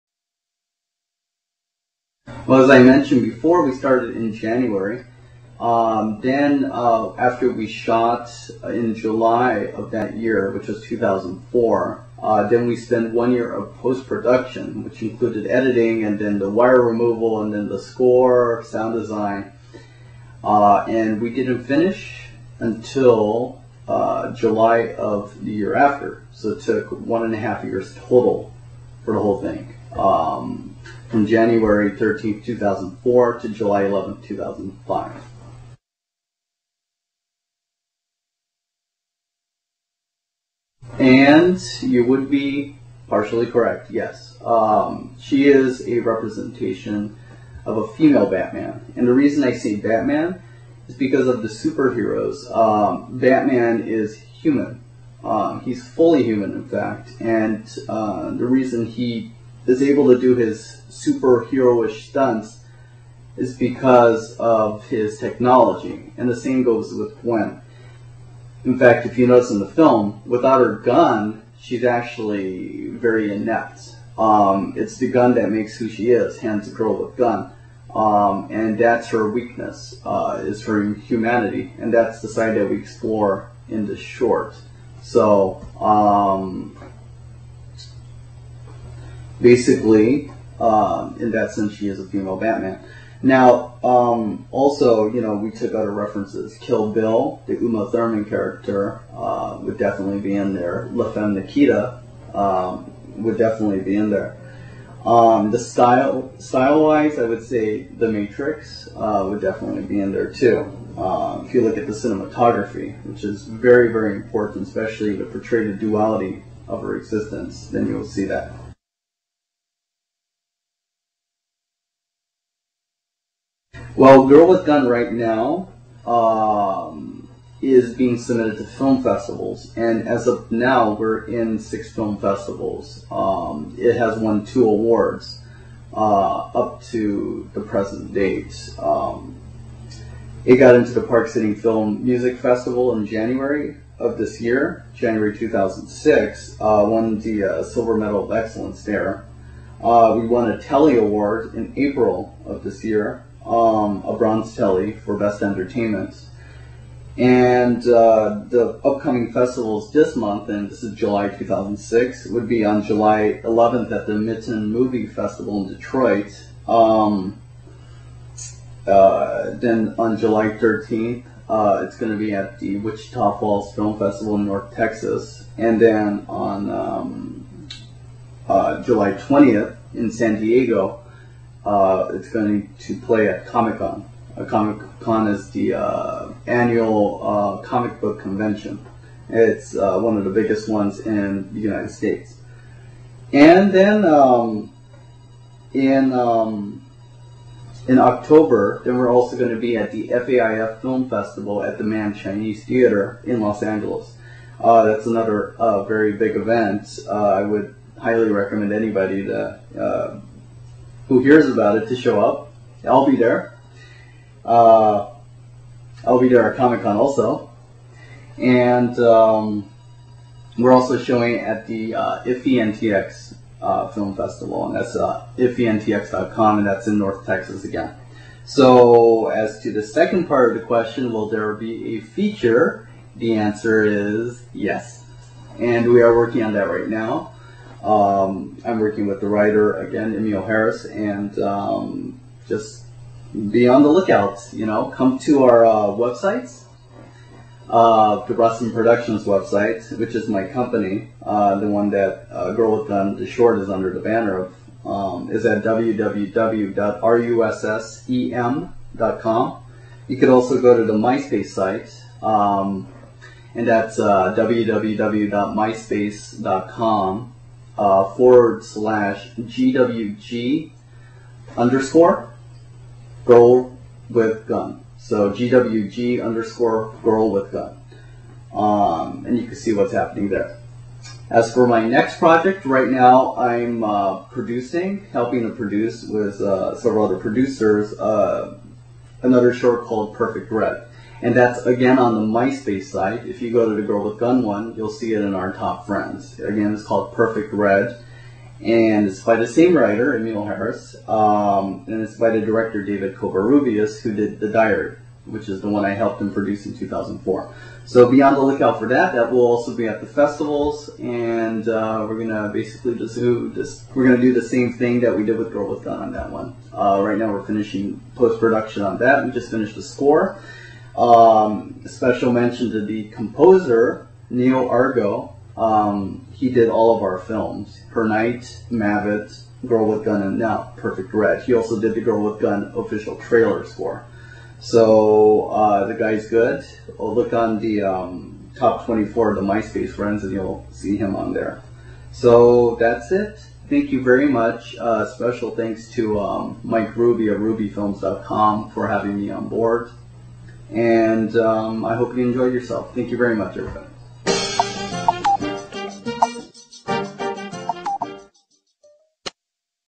Ruby Filmz Interview (Part 2)
Director Russ Emanuel is interviewed by Mike Ruby of Ruby Filmz - concerning "Girl With Gun".  Part 2 of 2.